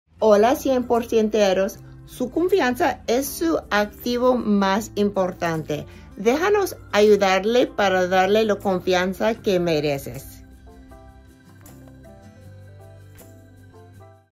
Descubre cómo New Medical Spa puede ayudarte a mejorar tu autoestima y confianza
¡Hola! Sabemos que la confianza es un recurso importante en todas las áreas de la vida, incluyendo su apariencia. En New Medical Spa, ofrecemos servicios de cuidado de la piel que lo ayudarán a sentirse mejor y aumentar su seguridad. Nuestros tratamientos como inyecciones de Botox, tratamientos faciales y masajes, son opciones excelentes para que mejore su apariencia y se sienta más seguro. Permítanos ayudarlo en su viaje de aumentar su autoconfianza. ¡Contáctenos hoy para obtener más información!